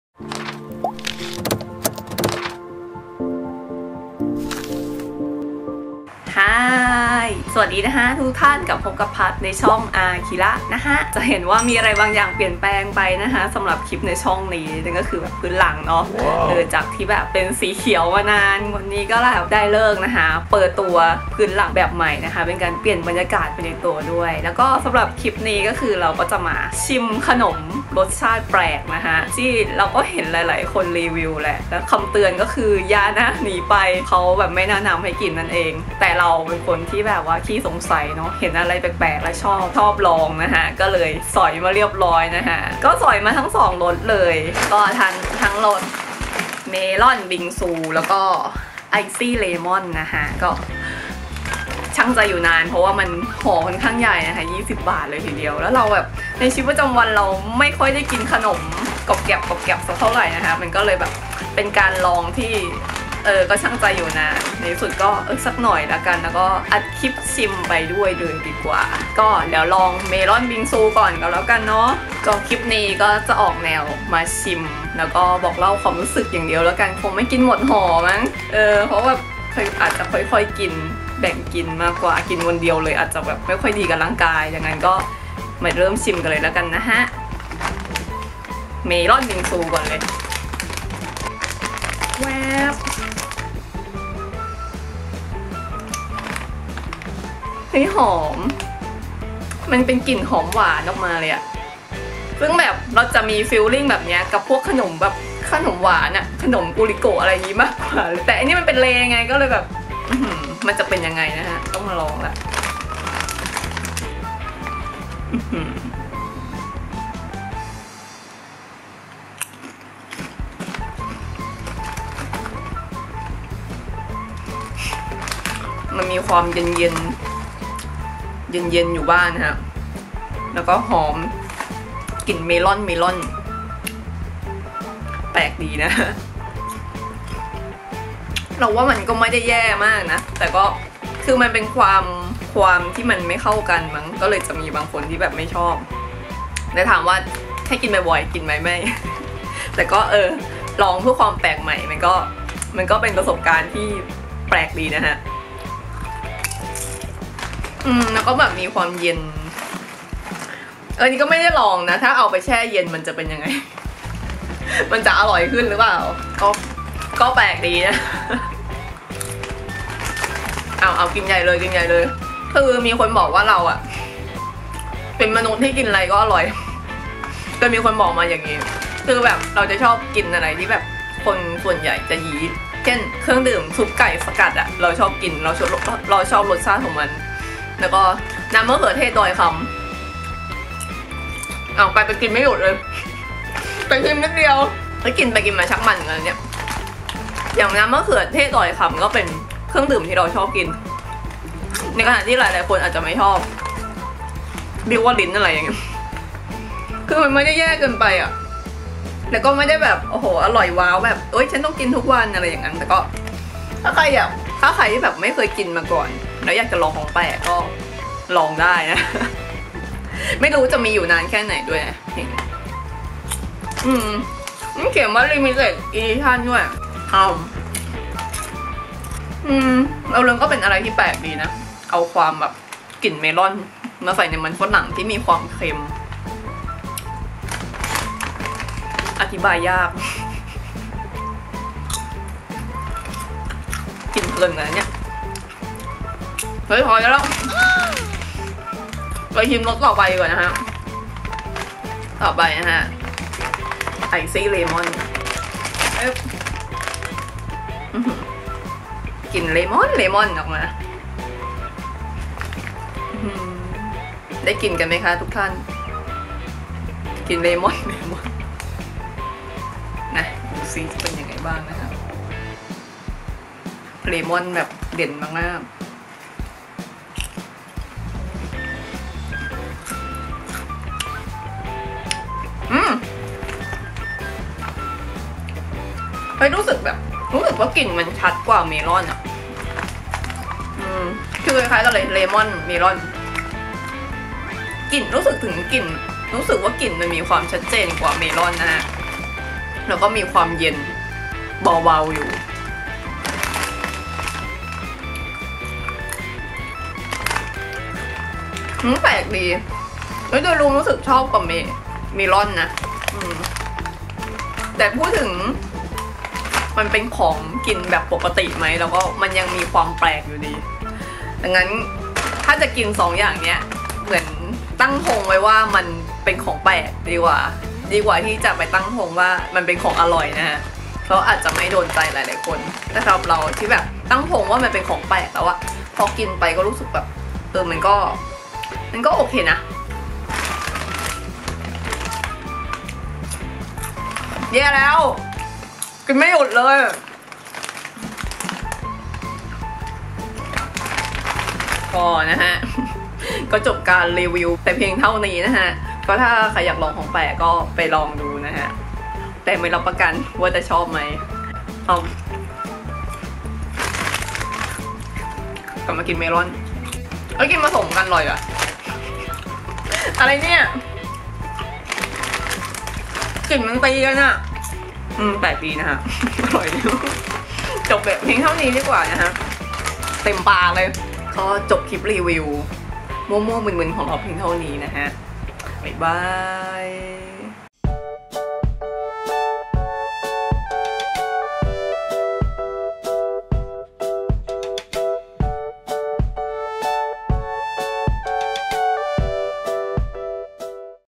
รีวิวเลย์ Cooling เมล่อนบิงซู + ไอซ์ซี่เลม่อน | ชิมไปบ่นไป | Archira
ได้ยินชื่อเสียงมานาน ขอลองสักหน่อย 555
จะเป็นอย่างไร ไปดู